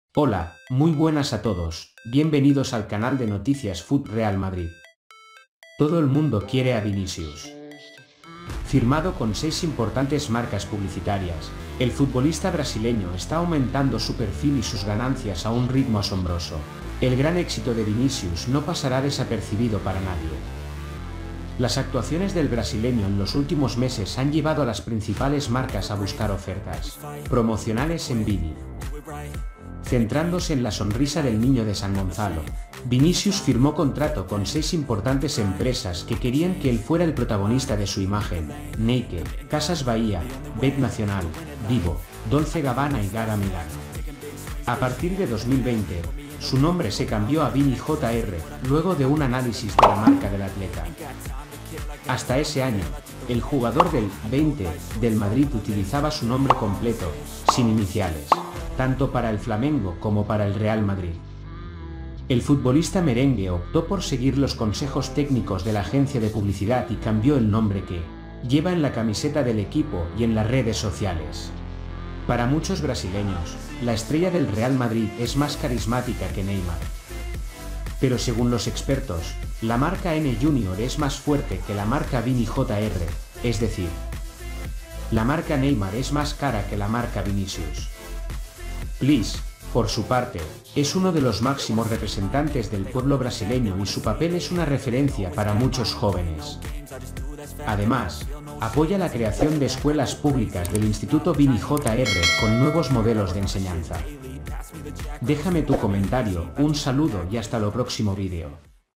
HA SALIDO AHORA| LO HAS VISTO ESO?|ULTIMA NOTICIA VINICIUS JR
HOLA ,muy buenas a TODOS ,BIEN VENIDOS AL CANAL  DE NOTICIAS FUT REAL MADRID
Todo el mundo quiere a Vinicius.
Firmado con seis importantes marcas publicitarias, el futbolista brasileño está aumentando su perfil y sus ganancias a un ritmo asombroso.
El gran éxito de Vinicius no pasará desapercibido para nadie. Las actuaciones del brasileño en los últimos meses han llevado a las principales marcas a buscar ofertas promocionales en Vini. Centrándose en la sonrisa del niño de San Gonzalo, Vinicius firmó contrato con seis importantes empresas que querían que él fuera el protagonista de su imagen: Nike, Casas Bahía, Bet Nacional, Vivo, Dolce Gabana y Gaga Milano.
A partir de 2020, su nombre se cambió a Vini Jr. luego de un análisis de la marca del atleta. Hasta ese año, el jugador del '20' del Madrid utilizaba su nombre completo (sin iniciales) tanto para el Flamengo como para el Real Madrid.
Para muchos brasileños, la estrella del Real Madrid es más carismática que Neymar. Pero según los expertos, la marca NJr es más fuerte que la marca Vini Jr, es decir. La marca Neymar es más cara que la marca Vinicius. Pliss, por su parte, es uno de los máximos representantes del pueblo brasileño y su papel es una referencia para muchos jóvenes. Además, apoya la creación de escuelas públicas del Instituto Vini Jr con nuevos modelos de enseñanza.
déjame tu comentario ,un saludo y hasta lo próximo vídeo